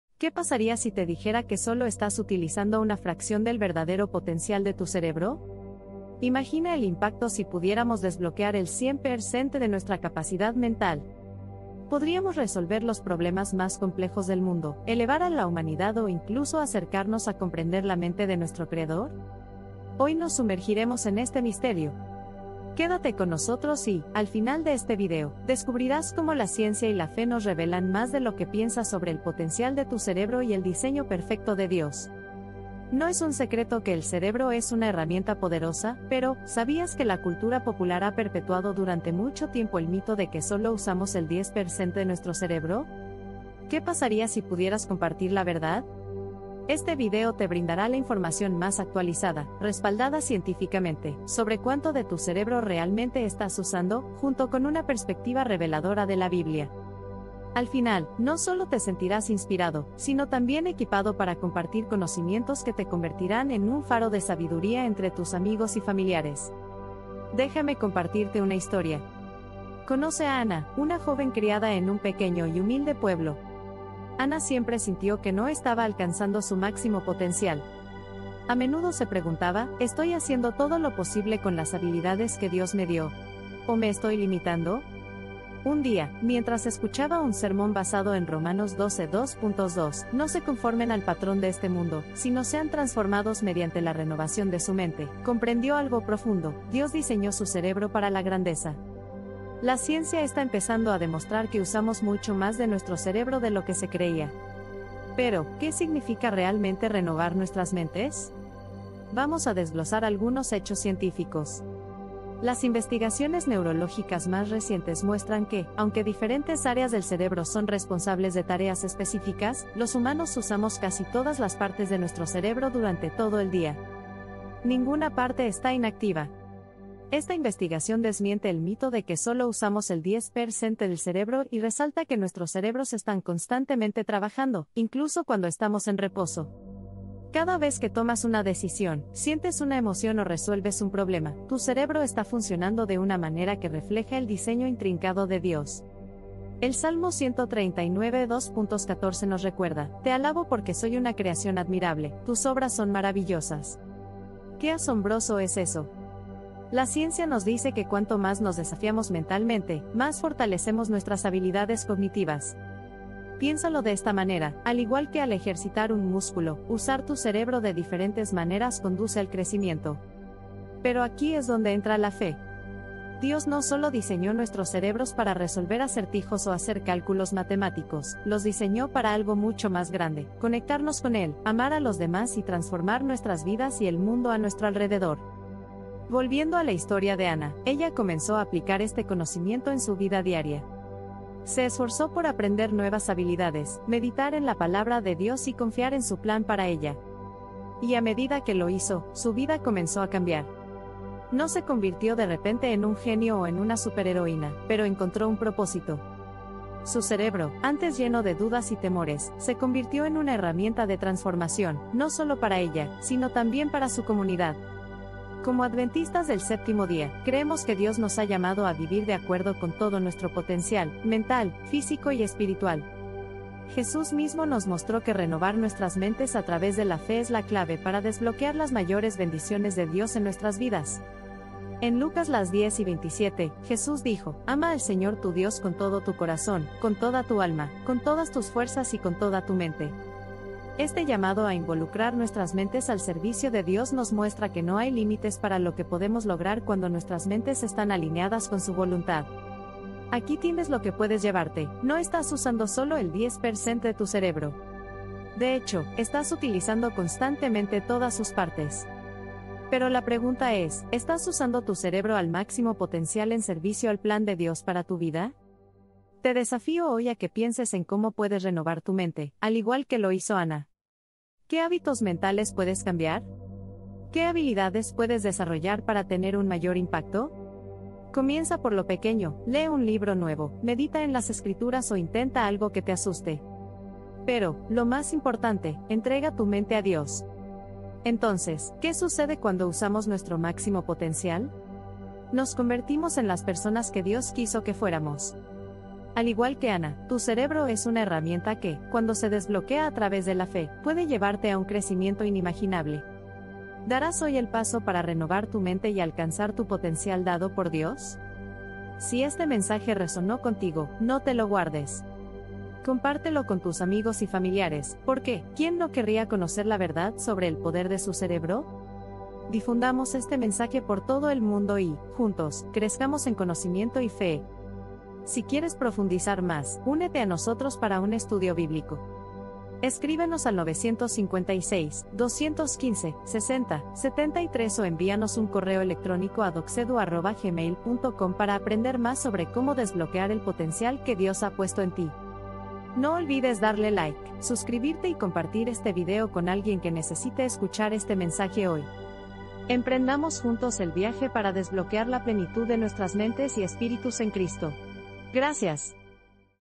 ¡DESBLOQUEA TU SUPER CEREBRO! Fe, Ciencia y el Poder Oculto en Ti
¿Y si te dijera que posees un potencial ilimitado dentro de tu mente? 🤔 En este viaje inspirador, exploramos la intersección entre la fe y la ciencia para desbloquear el verdadero poder de tu cerebro.
Prepárate para descubrir:
Técnicas milenarias y descubrimientos científicos de vanguardia para expandir tu mente. 🤯
Cómo la fe puede impulsar tu capacidad cerebral y ayudarte a superar cualquier obstáculo. 🙏
Historias reales de transformación personal y superación a través del poder del cerebro. 🚀
¡Descubre cómo desbloquear tu super cerebro con la combinación perfecta de fe, ciencia y el poder oculto en ti! Este video te mostrará cómo alcanzar tu máximo potencial mental y espiritual. ¡No te lo pierdas!
¡Únete a esta aventura de autodescubrimiento y libera el gigante que llevas dentro!